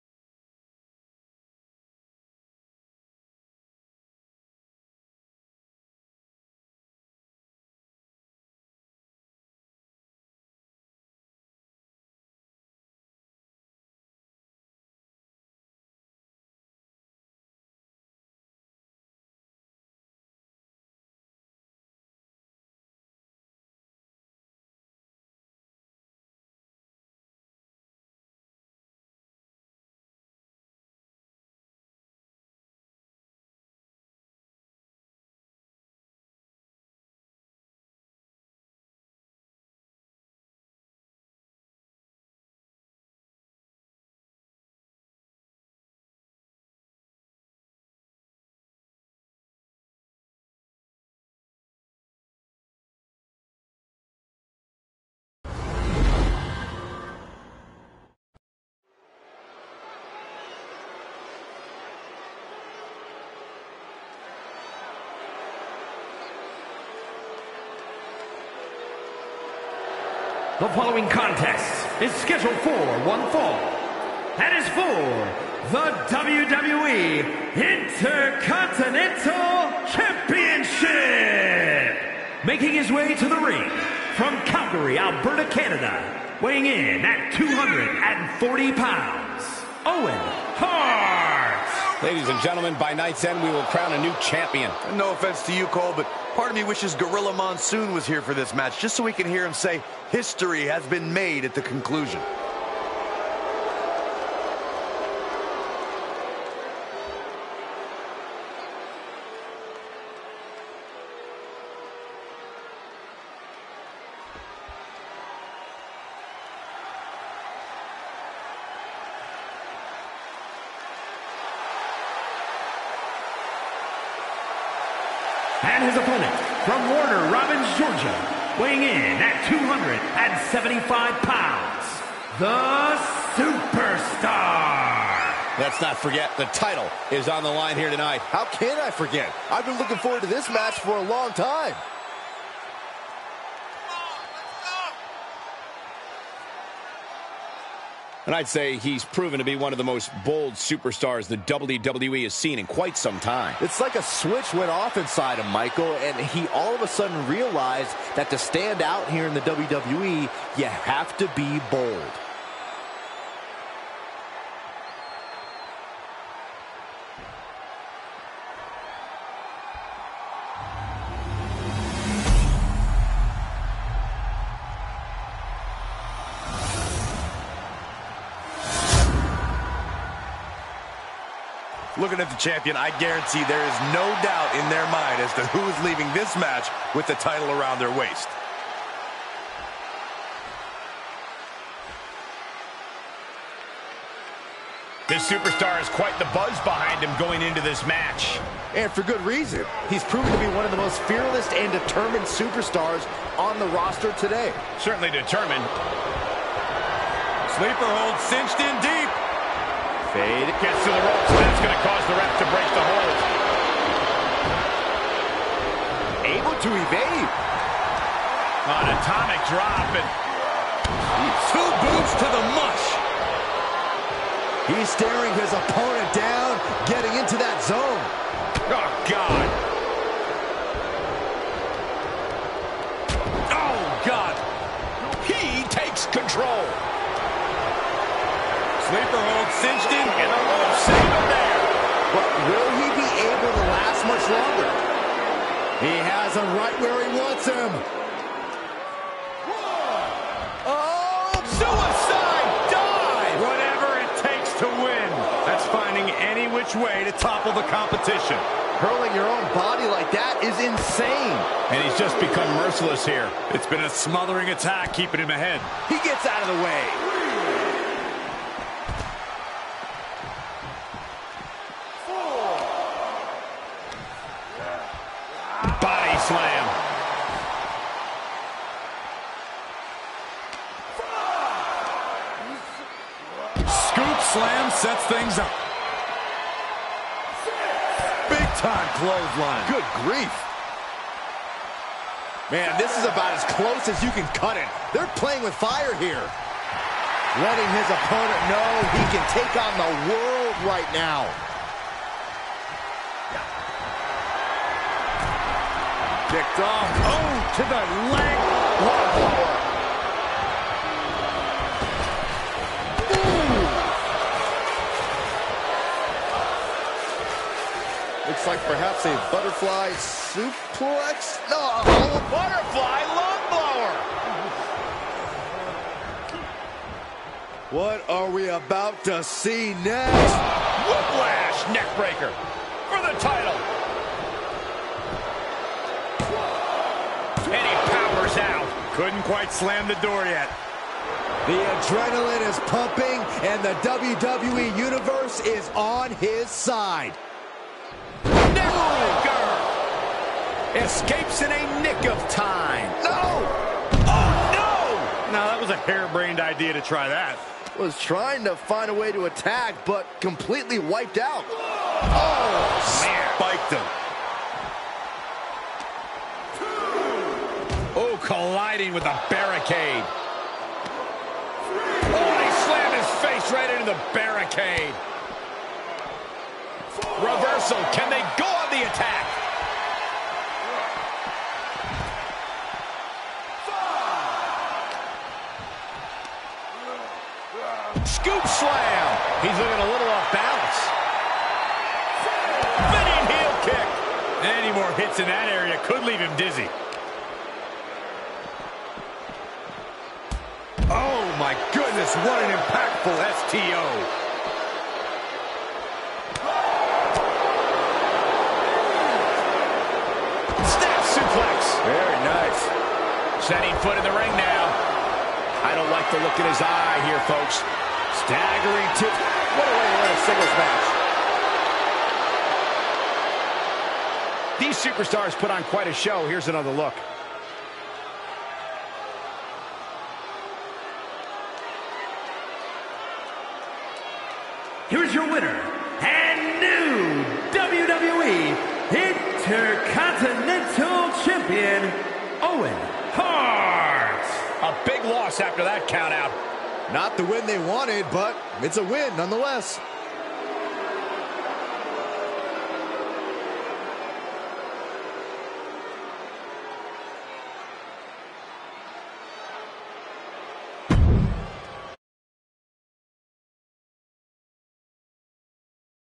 WWF In Your House 18: Badd Blood (Match 4) Owen Hart vs Faarooq
WWF In Your House 18: Badd Blood 1997, recreated and re-matched...  See all the wrestlers, all the matches, the FULL EVENT!!  A great night of wrestling with some really unforgettable matches, that you are sure to enjoy!